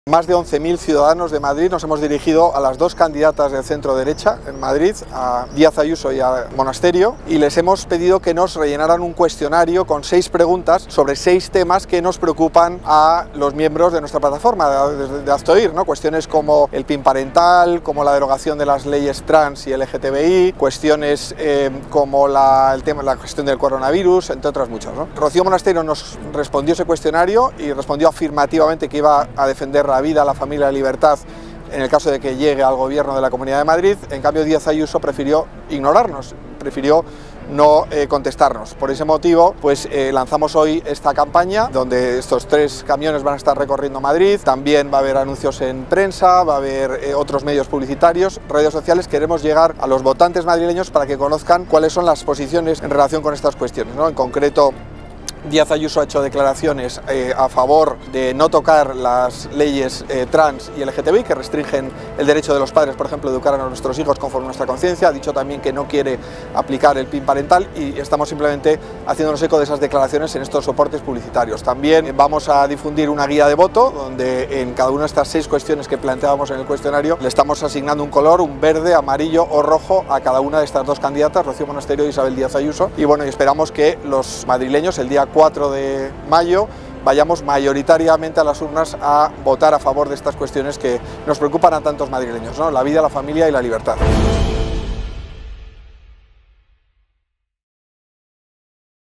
HazteOir.org presenta los vehículos 'Vota Valores' para las elecciones del 4M en Madrid
Ignacio Arsuaga ha presentado los vehículos informativos que muestran la inconsistencia ideológica de Isabel Díaz Ayuso y la Guía de Voto que comapara la posición de las dos formaciones políticas de centro-derecha para las elecciones autonómicas de Madrid.